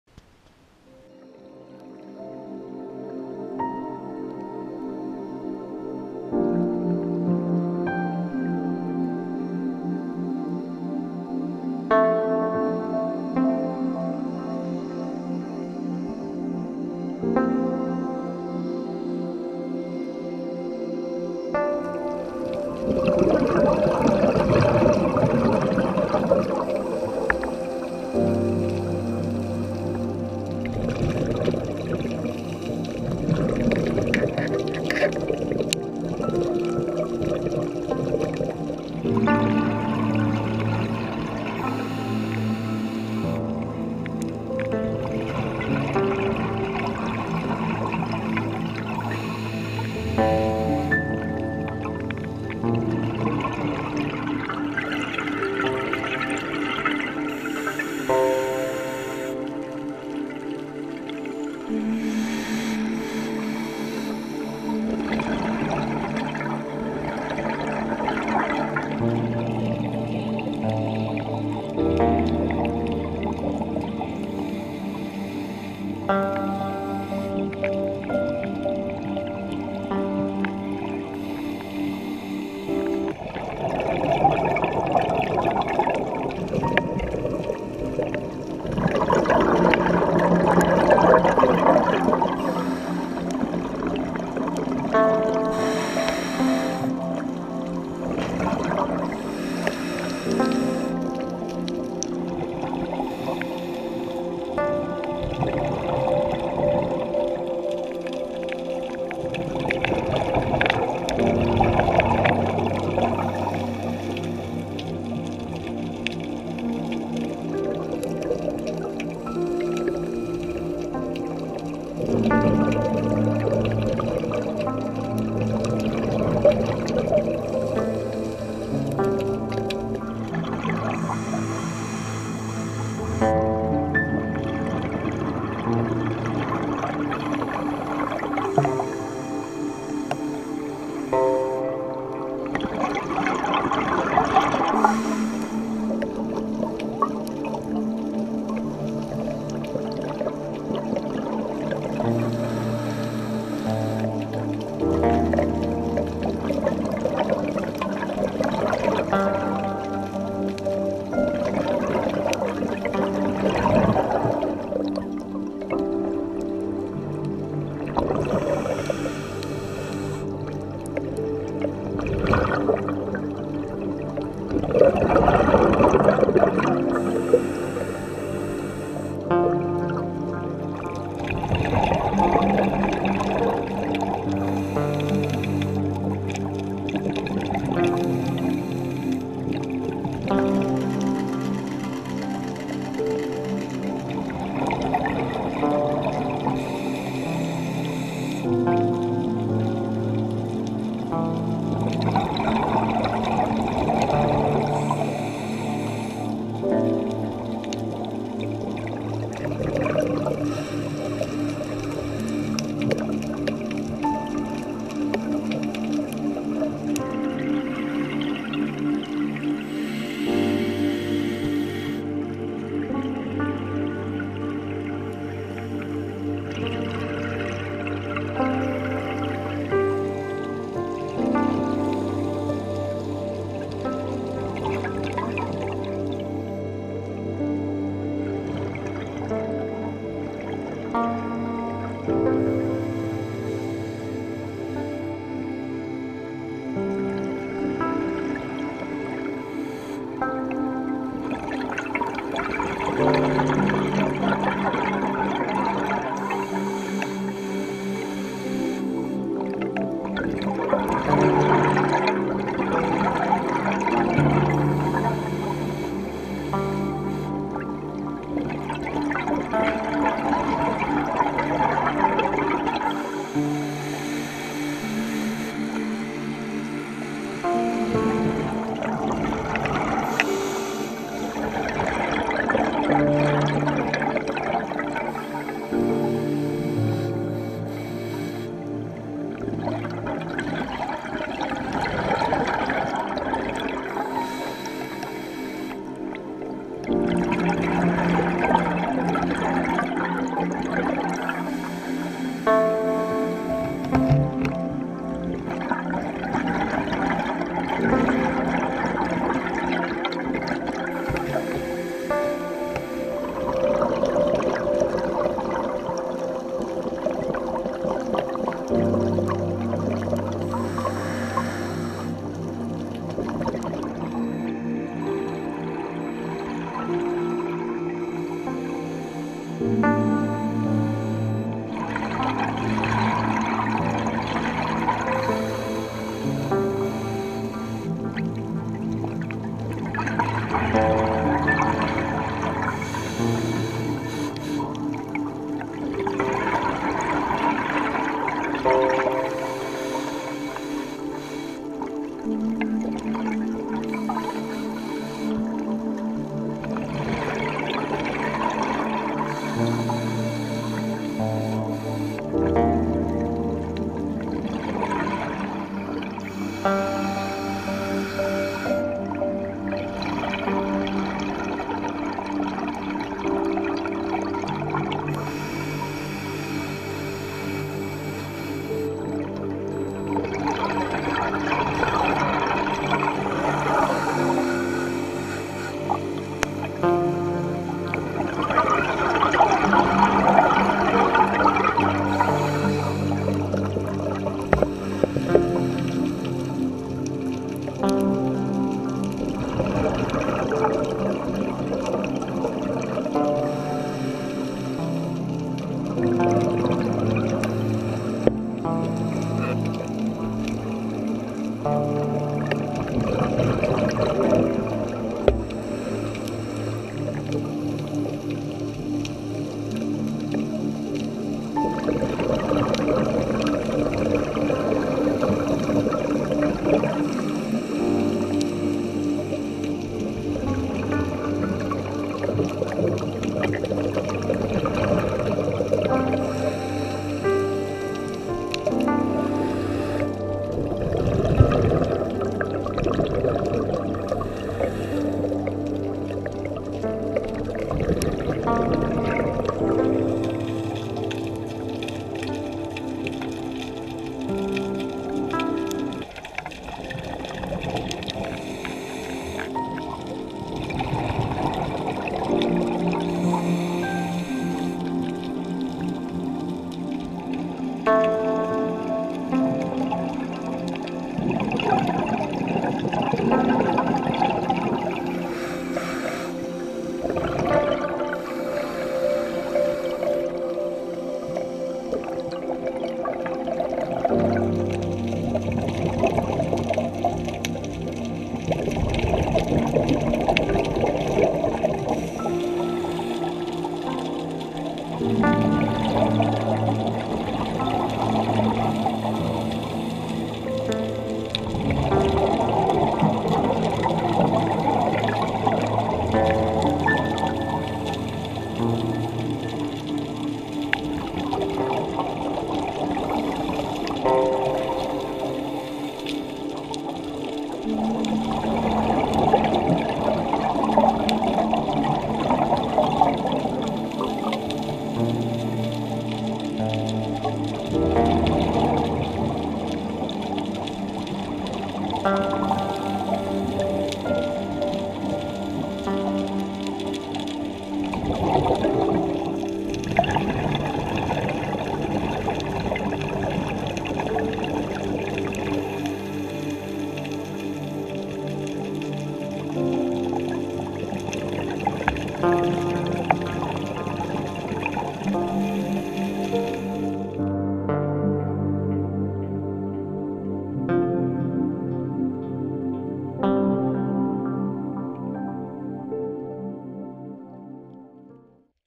Beneath the Mediterranean Villefranche sur Mer
A short video showing some of the underwater fauna and flora to be seen in Villefranche bay between Cap Nice and Cap Ferrat from shallow reefs down to 40m.